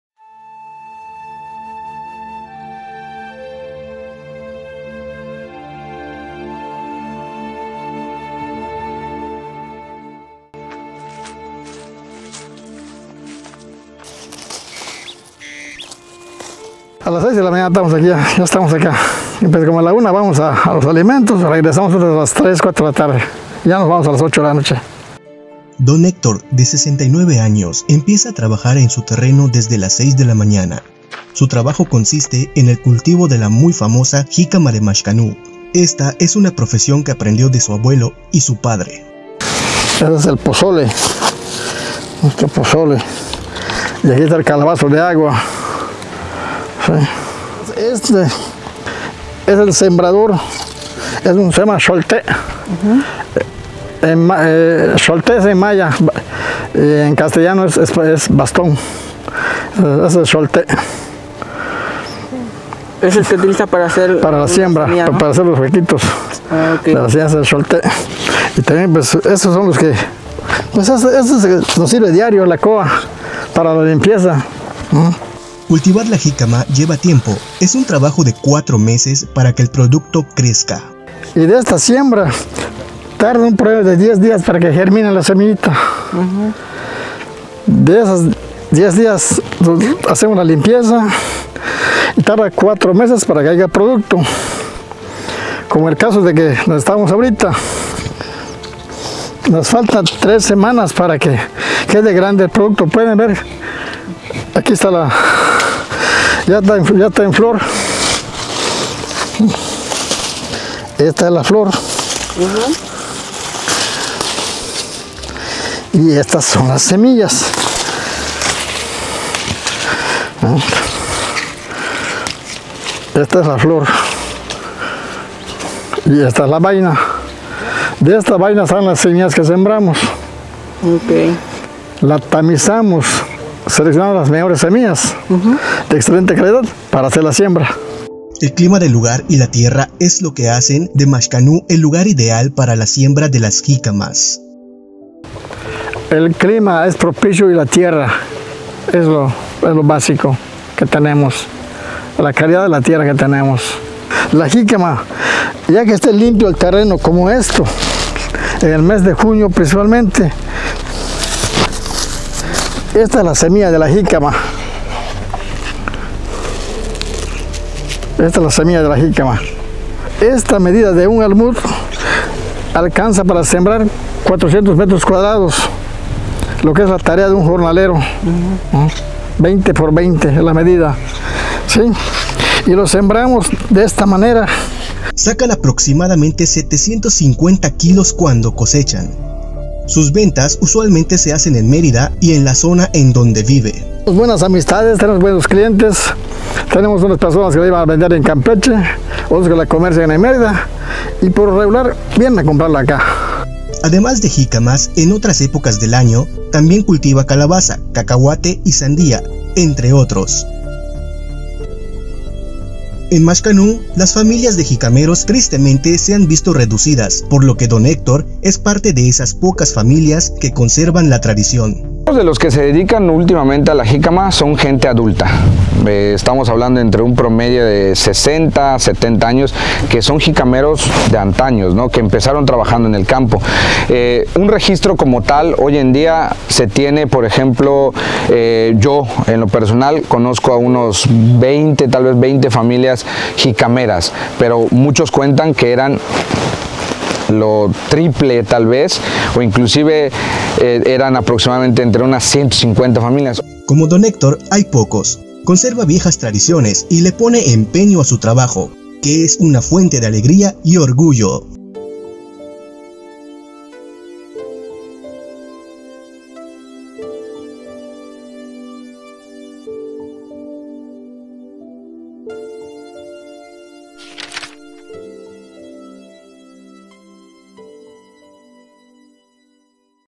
LOS JICAMEROS DE MAXCANÚ
En el municipio de Maxcanú, Yucatán, desde hace muchos años, personas del campo se dedican al cultivo de las jícamas, un tubérculo muy consumido en el país.

Señalan que las mejores Jícamas del sureste, salen de las tierras de Maxcanú. Don Héctor, un jicamero, nos cuenta los secretos para que la jícama crezca grande y sabrosa...